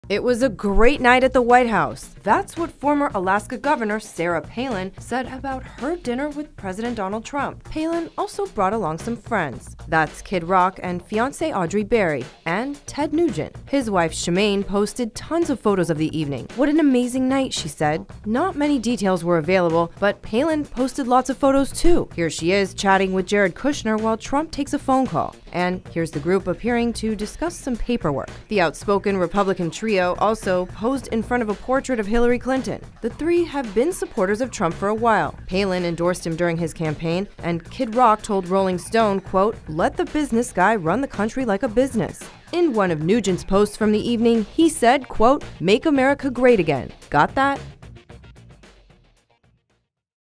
Sarah Palin Visits the White House with Kid Rock and Ted Nugent
Sarah Palin visits the White House with Kid Rock and Ted Nugent. Elizabeth Keatinge (@elizkeatinge) has more.